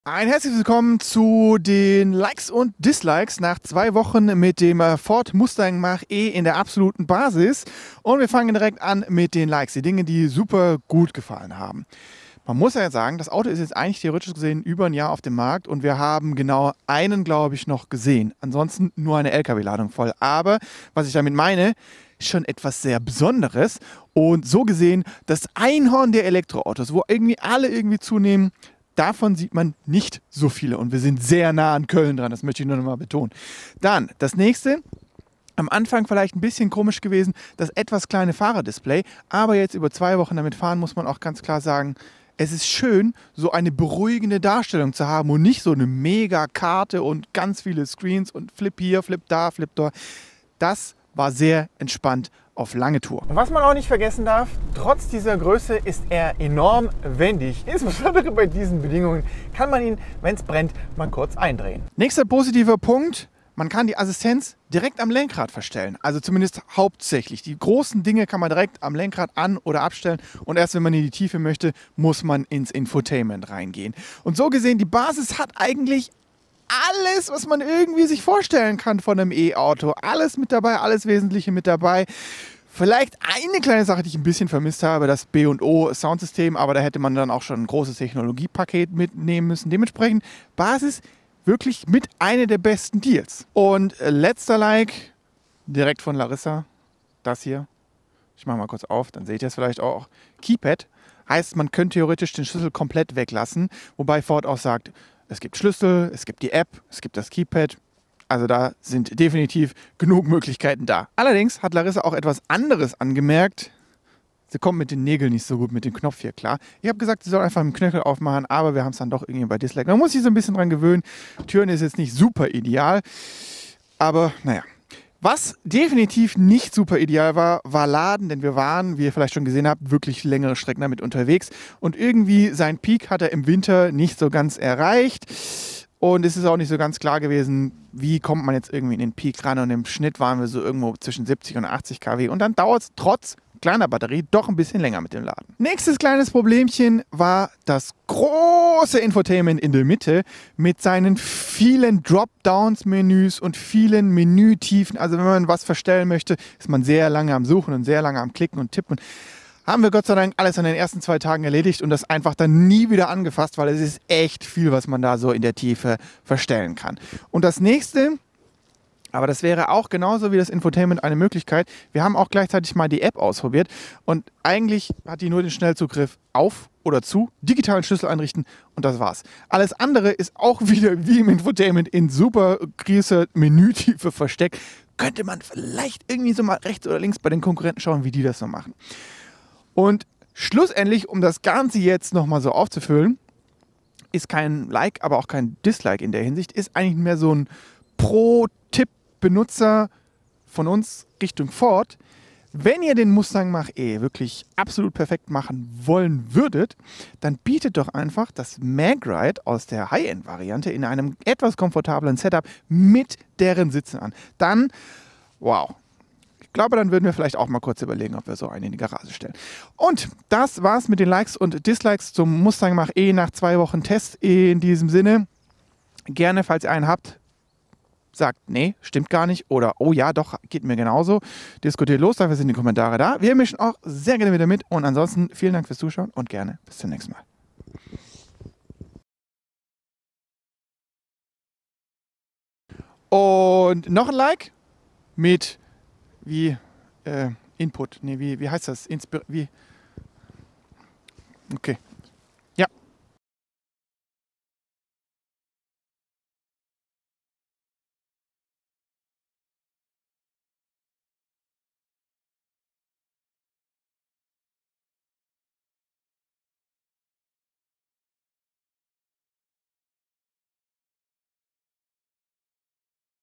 🔋 Mustang Mach-E: Was uns gefällt und was....Nicht [4K] - Autophorie Extra
Nach knapp zwei Wochen mit dem Mustang Mach-E SR haben sich schnell einige positive Punkte am Crossover Pony feststellen lassen. Aber auch ein paar Punkte, die einfach besser sein könnten. Und das wären nicht mal schwer zu ändernde Punkte :)